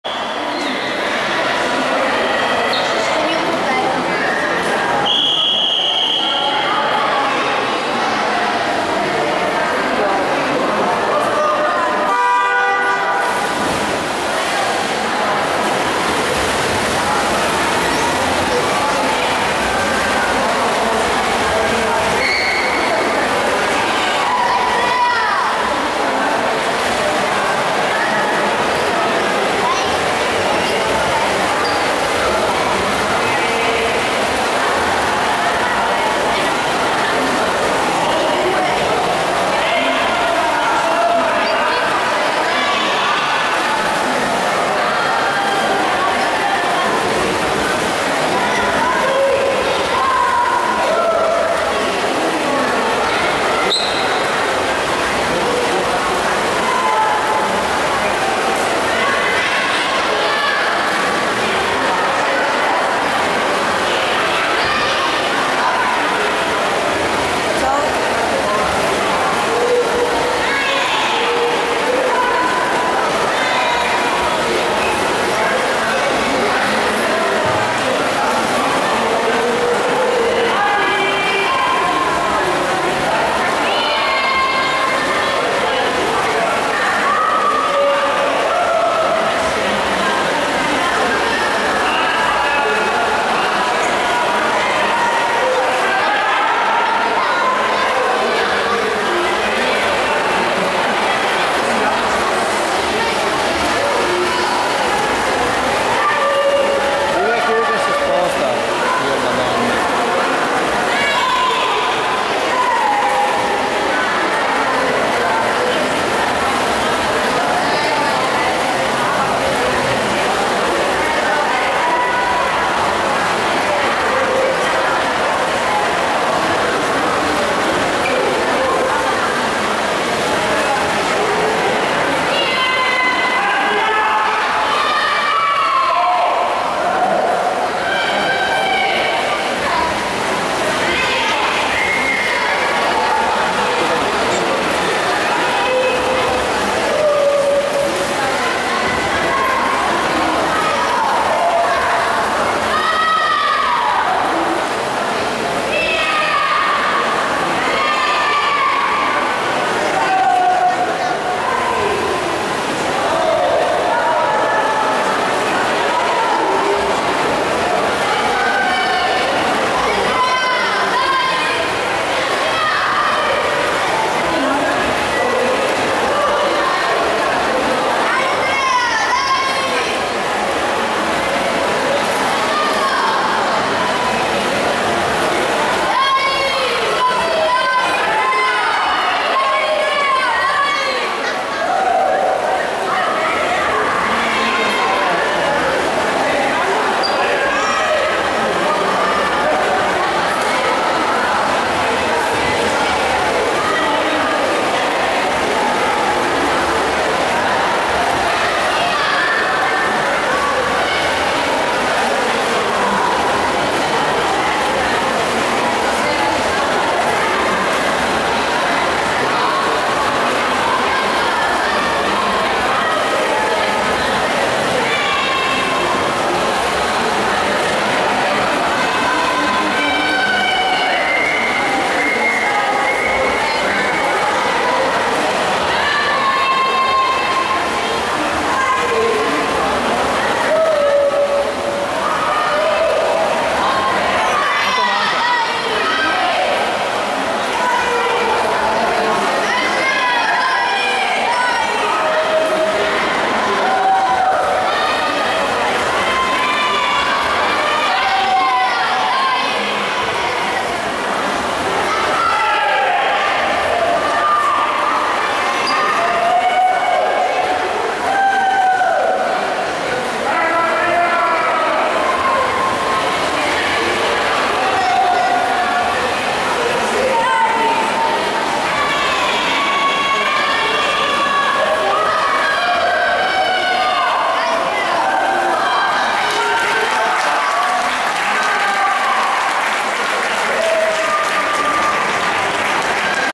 THOMAS CECCON Esordienti A 1 anno ( 12 yars old) 400 free 4.38.1 lane 3
centro nuoto Palladio Vicenza il 9.6.2013